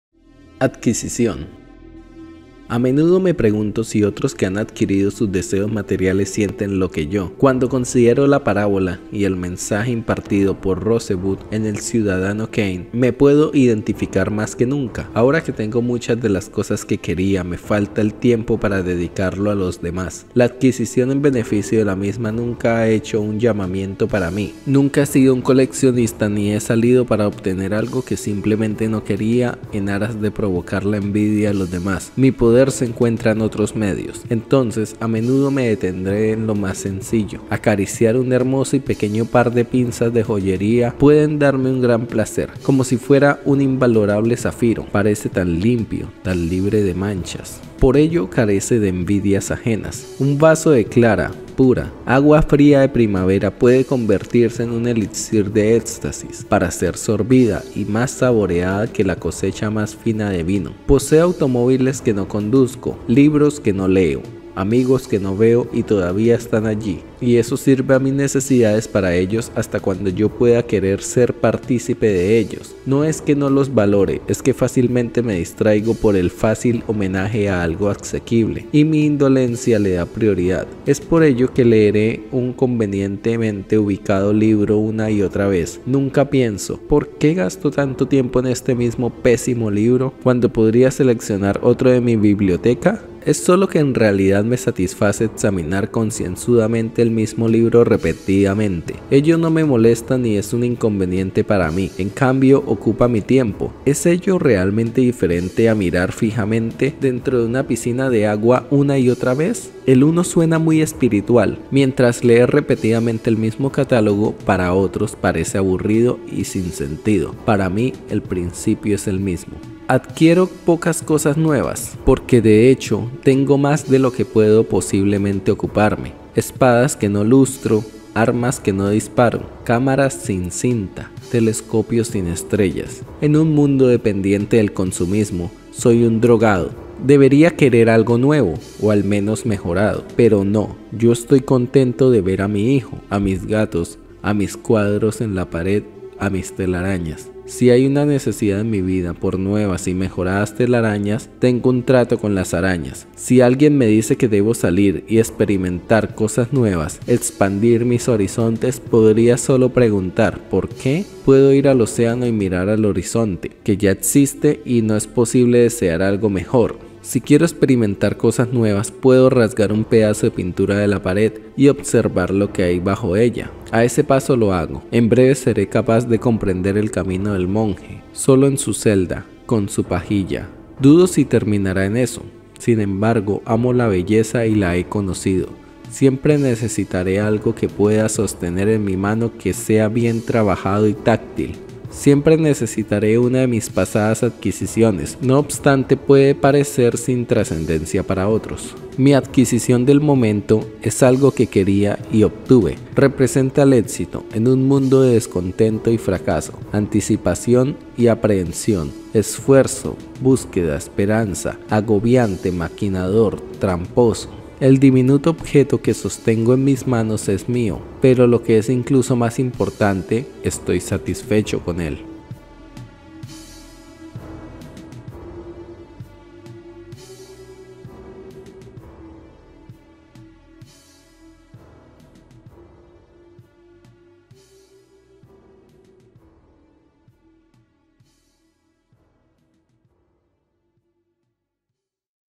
Adquisición | Satán Habla (Anton Szandor Lavey) audio libro
Adquisición, por Anton Szandor Lavey.

En este ensayo del libro habla satán, Lavey da ejemplos de como es que muchas veces adquirimos cosas que jamás utilizamos, o que usamos una sola vez, y al final incluso hasta olvidamos; todo porque vivimos en un mundo en consumista.